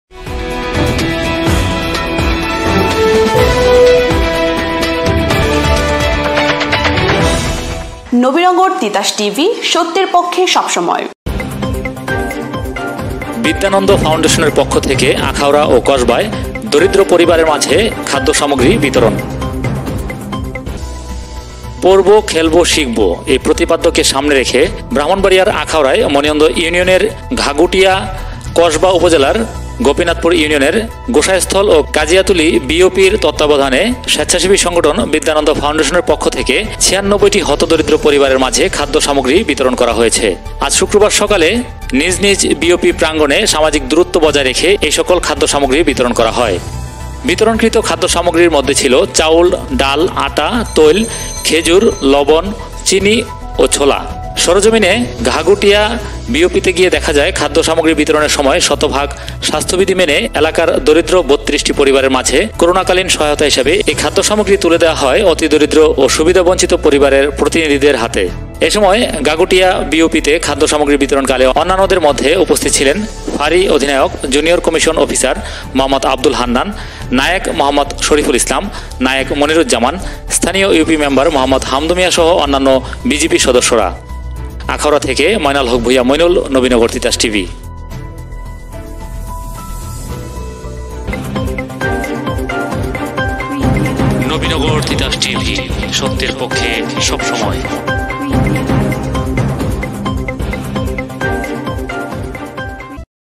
বিদ্যানন্দ ফাউন্ডেশনের পক্ষ থেকে দরিদ্র পরিবারের মাঝে খাদ্য সামগ্রী বিতরণ
পড়ব, খেলব, শিখব এই পতিপাদ্যকে সামনে রেখে ব্রাহ্মণবাড়িয়ার আখাউড়ায় মনিয়ন্দ ইউনিয়নের ঘাগুটিয়া, কসবা উপজেলার গোপীনাথপুর ইউনিয়নের গোসাইস্থল ও কাজিয়াতুলী বিওপির তত্বাবধানে স্বেচ্ছাসেবী সংগঠন বিদ্যানন্দ ফাউন্ডেশনের পথেকে ৯৬টি হত দরিদ্র পরিবারের মাঝে খাদ্য সামগ্রী বিতরন করা হয়েছে।

আজ শুক্রবার সকাল ১০টা থেকে ১১টা মধ্যে নিজ নিজ বিওপি প্রাঙ্গণে সামাজিক দুরত্ব বজায় রেখে এই সকল খাদ্য সামগ্রী বিতরণ করা হয়। বিতরণকৃত খাদ্য সামগ্রীর মধ্যে ছিল, চাউল, ডাল, আটা, তেল, , খেজুর, লবন, চিনি, ছোলা ইত্যাদি।

সরোজমিনে ঘাগুটিয়া বিওপিতে গিয়ে দেখা যায়, খাদ্য সামগ্রী বিতরনের সময় শতভাগ স্বাস্থ্যবিধি মেনে এলাকার দরিদ্র ৩২টি পরিবারের মাঝে করোনাকালীন সহায়তা হিসেবে এই খাদ্য সামগ্রী তুলে দেওয়া হয় অতি দরিদ্র ও সুবিধাবঞ্চিত পরিবারের প্রতিনিধিদের হাতে।

এসময় ঘাগুটিয়া বিওপিতে খাদ্য সামগ্রী বিতরণকালে অন্যান্যদের মধ্যে উপস্থিত ছিলেন ফাড়ি অধিনায়ক  জুনিয়র কমিশন অফিসার মোঃ আব্দুল হান্নান, নায়েক মোঃ শরিফুল ইসলাম, নায়েক মনিরউজ্জামান, স্থানীয় ইউপি মেম্বার মোঃ হামদু মিয়াসহ অন্যান্য বিজিবি সদস্যরা।